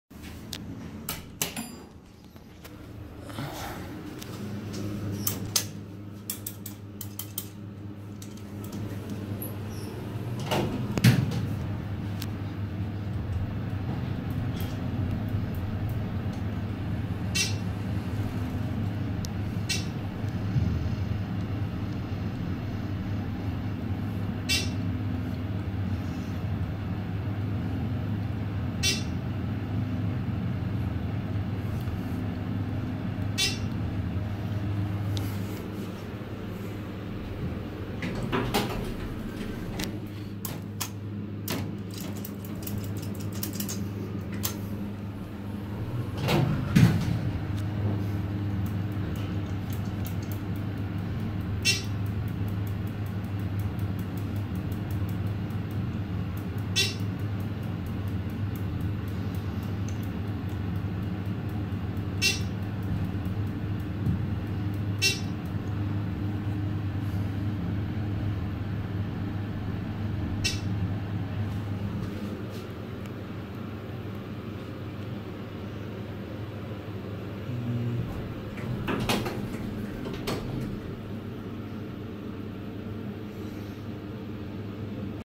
1994 Dover Hydraulic Right Elevator @ Bellevue Club Hotel, Bellevue WA
As of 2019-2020, their motors got replaced.

Taken 24 December 2018

Installed 1994